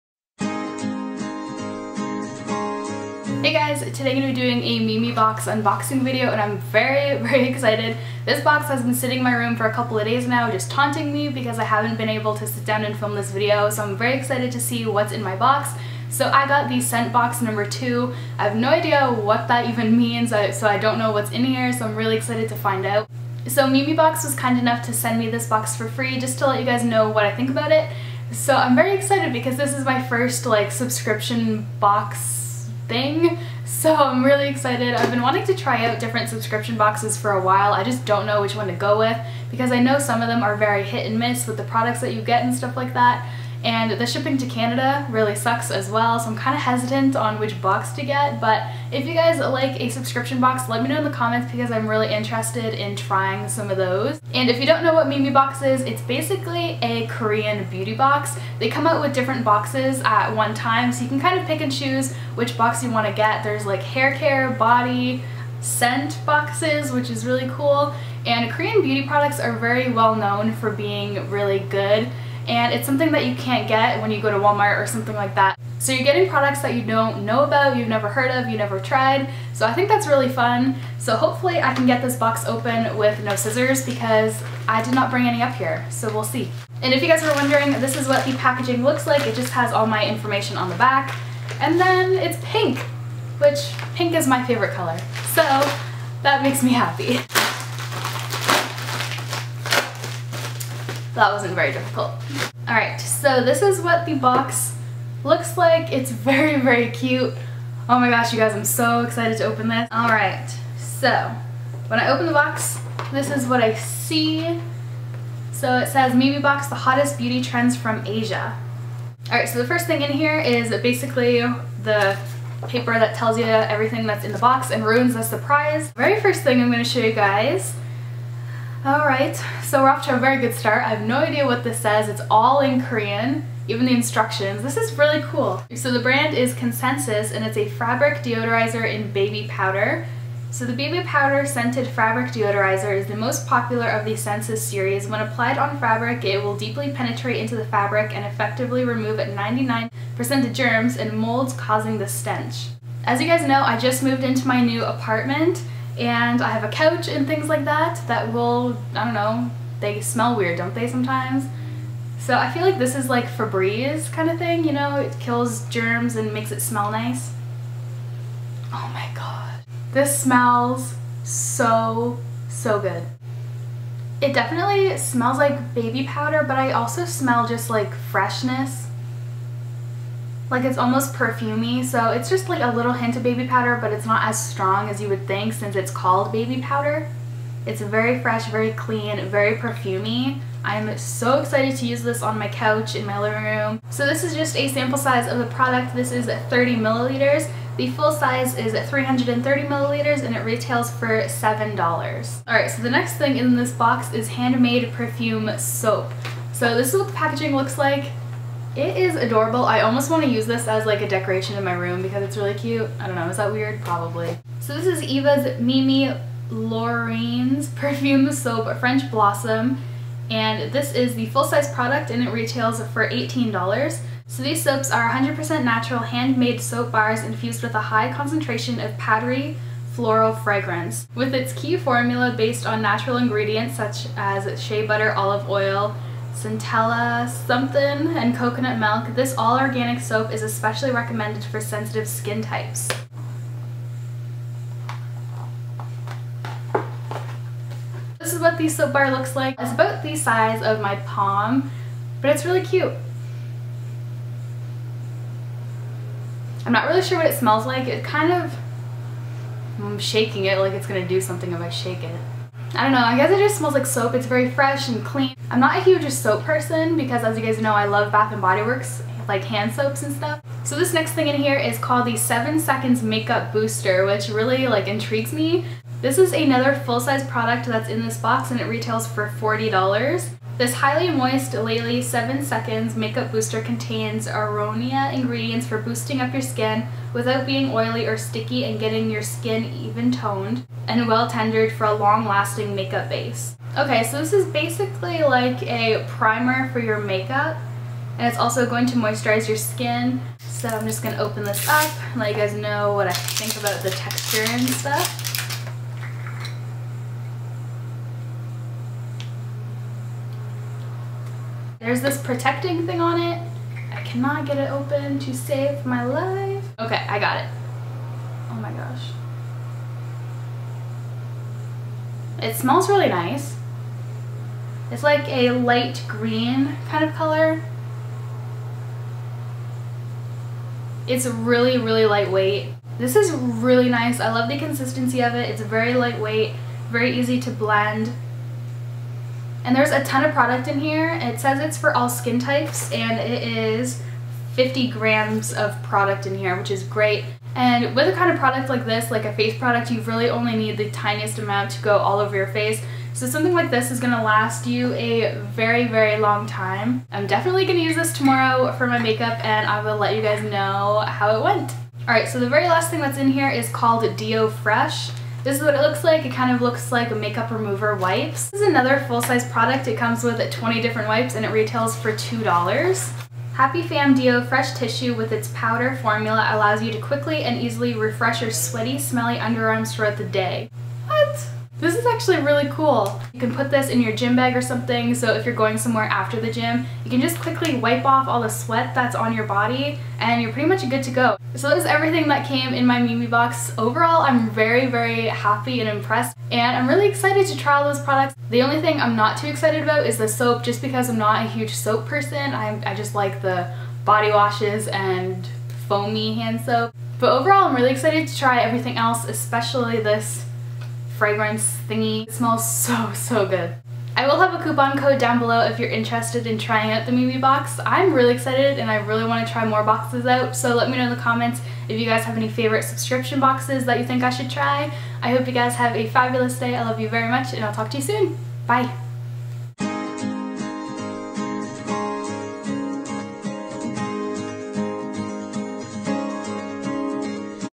Memebox Unboxing {Scent Box}
Hello lovelies! I am very excited to share my first Memebox with you! I hope you enjoy this video!

What I use to Edit: iMovie
Camera: Canon Rebel T3i
Lens: Kit lens & Canon 50mm
* I have permission to use this song.
Stay Beautiful.♥